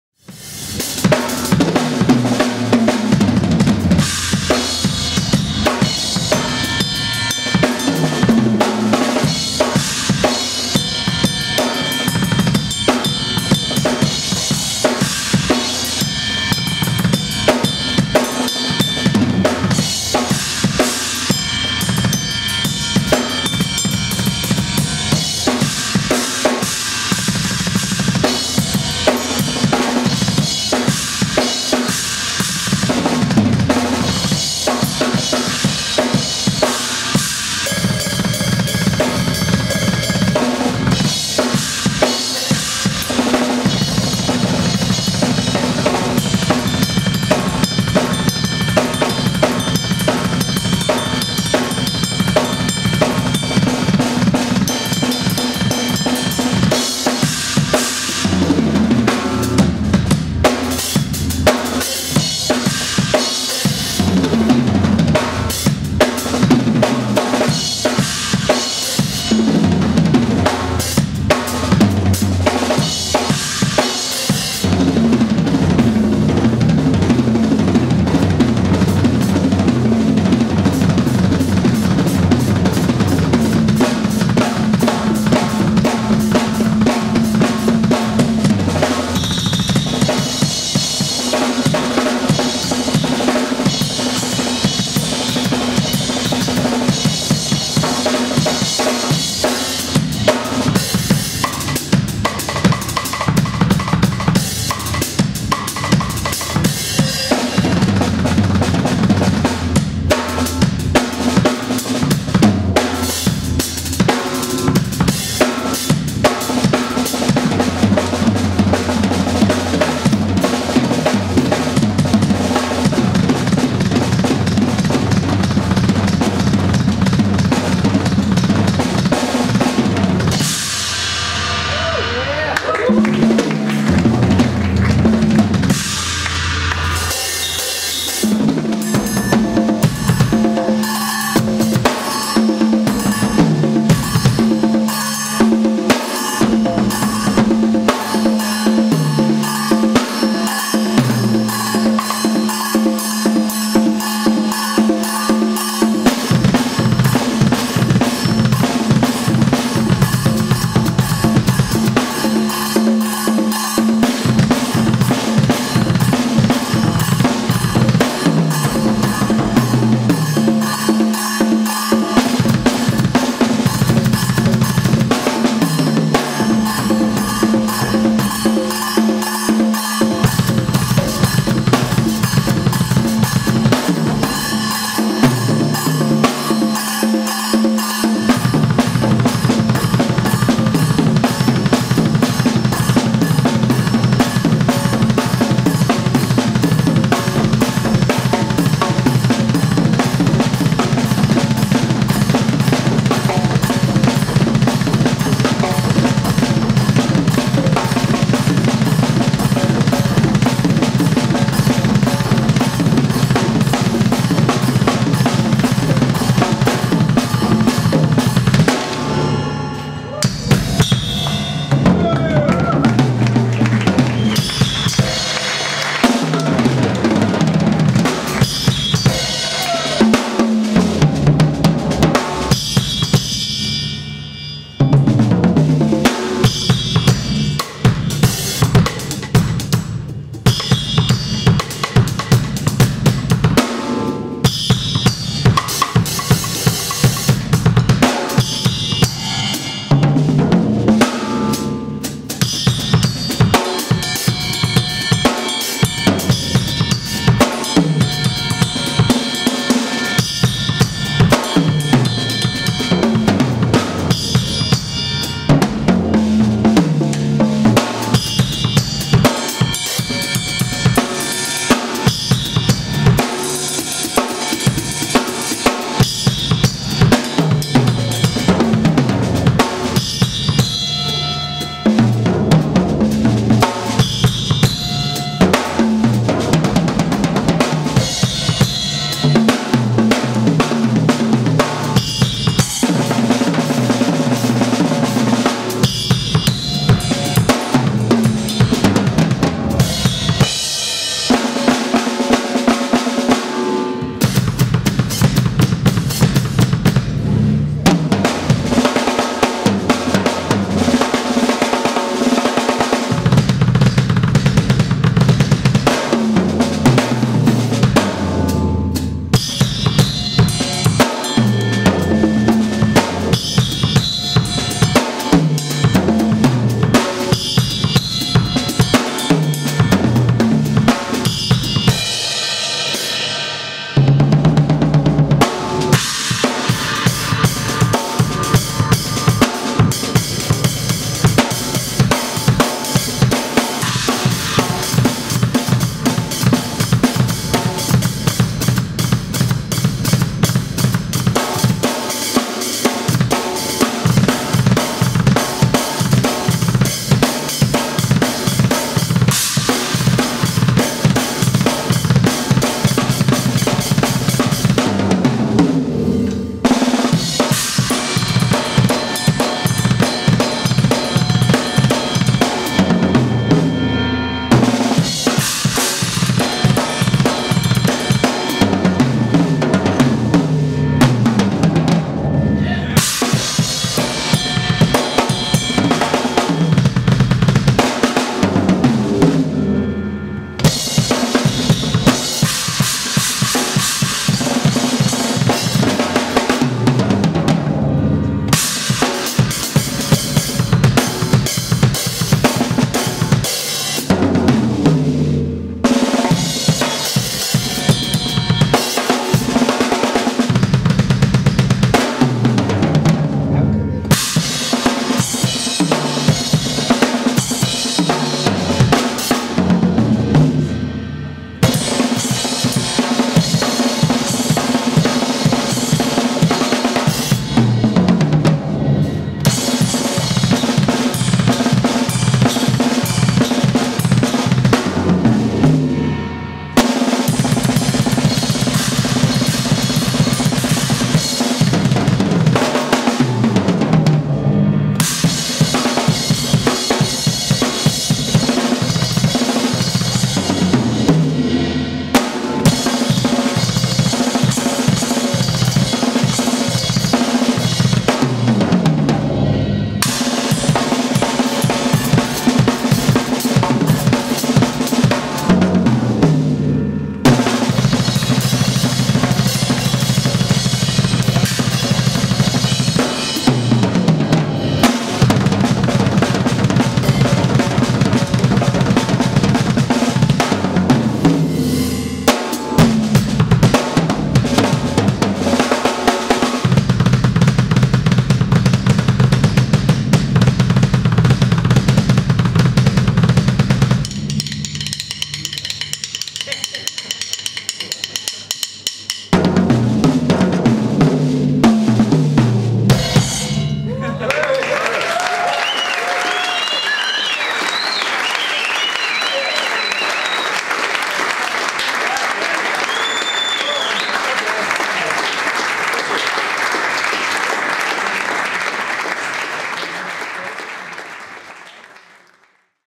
Marco Minnemann: Drum Clinic "Pro Percussion" - PART I - Basel - Switzerland
filmed by Bernhard Castiglioni - Drummerworld -
 Marco Minnemann: Drum Clinic "Pro Percussion" - PART I
Basel - Switzerland - 2010
More Videos here at DRUMMERWORLD